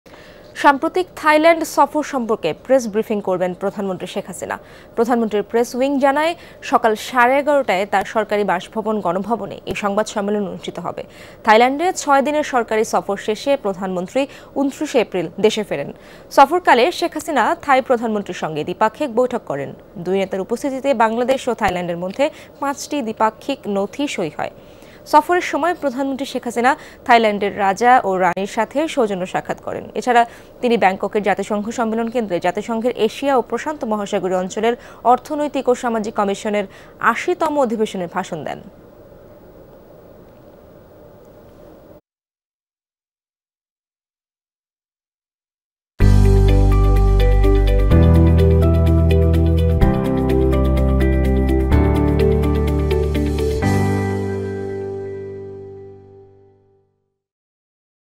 সাম্প্রতিক থাইল্যান্ড সফর সম্পর্কে প্রেস ব্রিফিং করবেন প্রধানমন্ত্রী শেখ হাসিনা | PM | Jamuna TV
সাম্প্রতিক থাইল্যান্ড সফর সম্পর্কে প্রেস ব্রিফিং করবেন প্রধানমন্ত্রী শেখ হাসিনা

প্রধানমন্ত্রীর প্রেস উইং জানায়, সকাল সাড়ে ১১টায় তার সরকারি বাসভবন- গণভবনে এই সংবাদ সম্মেলন অনুষ্ঠিত হবে। থাইল্যান্ডে ৬ দিনের সরকারি সফর শেষে; প্রধানমন্ত্রী ২৯ এপ্রিল দেশে ফেরেন। সফরকালে, শেখ হাসিনা থাই প্রধানমন্ত্রীর সঙ্গে দ্বিপাক্ষিক বৈঠক করেন। দুই নেতার উপস্থিতিতে বাংলাদেশ ও থাইল্যান্ডের মধ্যে ((ভিসা অব্যাহতি, জ্বালানি সহযোগিতা, পর্যটন ও শুল্ক সংক্রান্ত বিষয় এবং মুক্ত বাণিজ্য চুক্তির আলোচনার বিষয়ে)) পাঁচটি দ্বিপাক্ষিক নথি সই হয়। সফরের সময় প্রধানমন্ত্রী শেখ হাসিনা,থাইল্যান্ডের রাজা ((মহা ভাজিরালংকর্ন ফ্রা ভাজিরা-ক্লাওচা-উয়ুয়া)) ও রাণীর ((সুথিদা বজ্রসুধা-বিমলা-লক্ষণের)) সাথে সৌজন্য সাক্ষাৎ করেন। এছাড়া,  তিনি ব্যাংককের জাতিসংঘ সম্মেলন কেন্দ্রে জাতিসংঘের এশিয়া ও প্রশান্ত মহাসাগরীয় অঞ্চলের অর্থনৈতিক ও সামাজিক কমিশনের ৮০তম অধিবেশনে ভাষণ দেন।